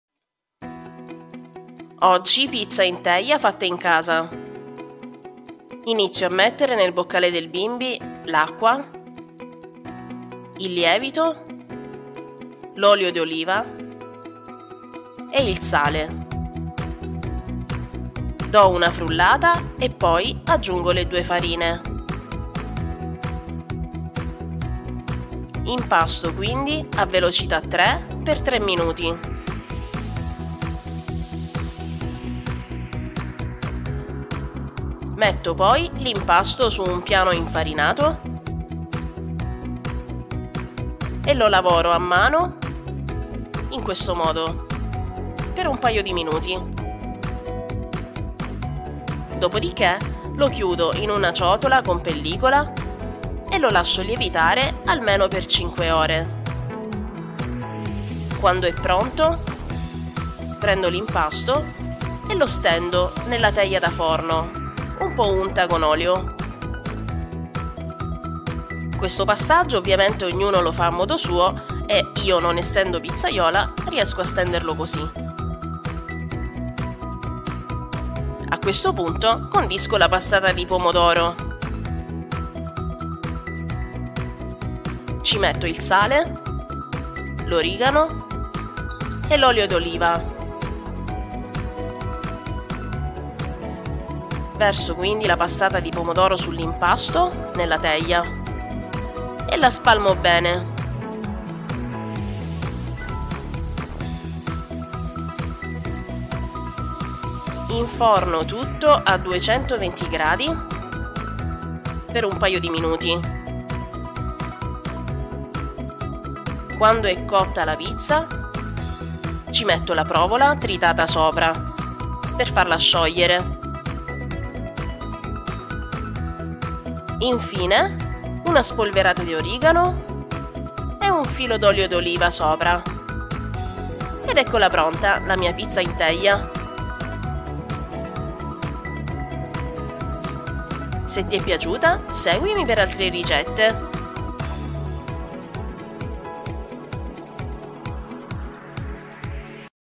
RICETTA: PIZZA in TEGLIA da FORNO, FATTA IN CASA. IMPASTO VELOCE per CENA, di NONNA Lina, con BIMBY
Deliziosa pizza in teglia fatta in casa con questa ricetta facile e veloce con bimby! Segui passo dopo passo per ottenere una pizza soffice e croccante, perfetta per ogni occasione. Utilizzo ingredienti semplici e genuini per un risultato sorprendente. Ideale per una cena in famiglia o con amici!  🍕

INGREDIENTI E DOSI:
- 250 gr. di farina Caputo "Pizzeria"
- 250 gr. di farina Caputo "Nuvola"
- 8 gr. di lievito di birra
- 350 ml di acqua
- 30 gr. di olio d'oliva